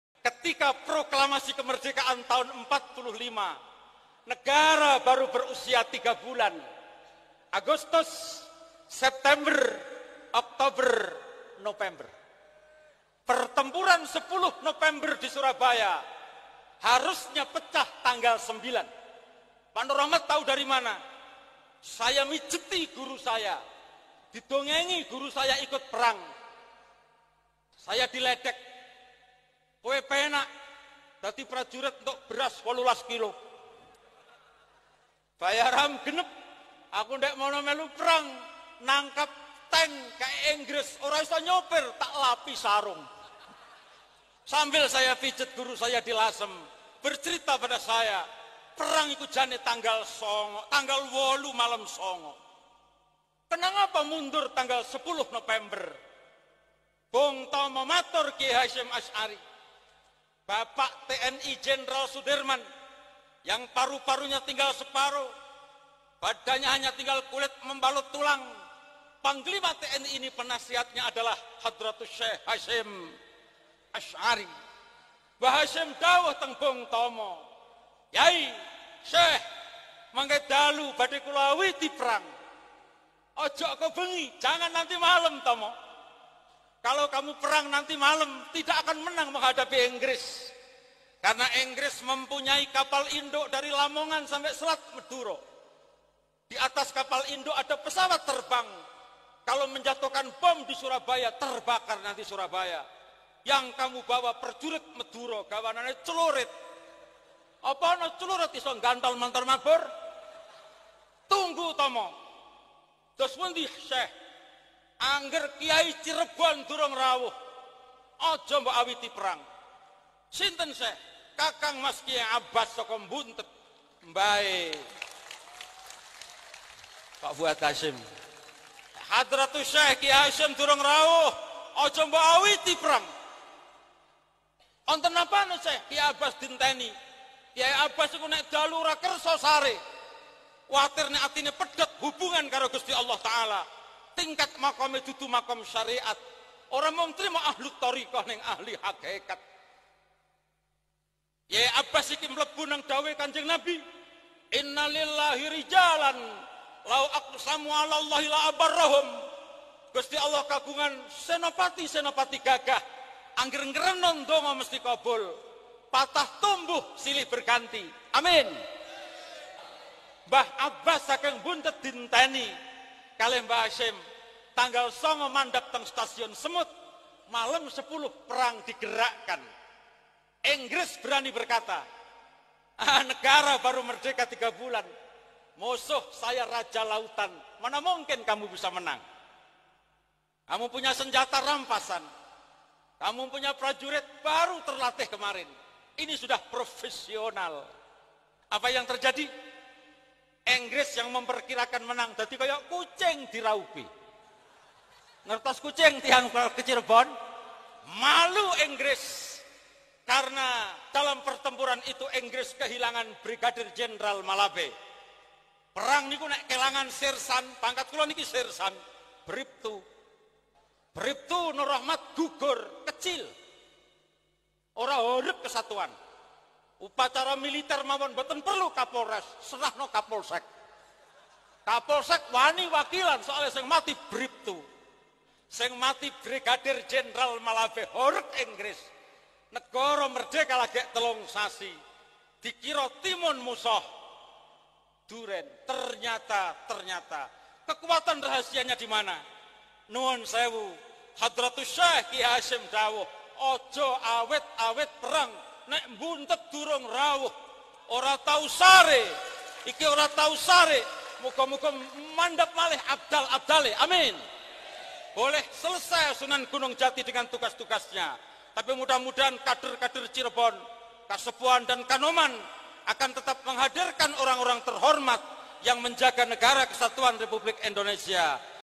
KH. ABBAS BUNTET PESANTREN CIREBON KOMANDO PERANG
Perang di Surabaya tanggal 10 November.. perang melawan Inggris yang membonceng setelah Indonesia merdeka. Bangsa Indonesia gigih mempertahankan kemerdekaan. Pertempuran di Surabaya, dikomandoi oleh seorang ulama beliau adalah kyai Haji Abbas Abdul Jamil dari Buntet Cirebon didampingi oleh bung Tomo peserta prajurit para santri arek-arek Suroboyo.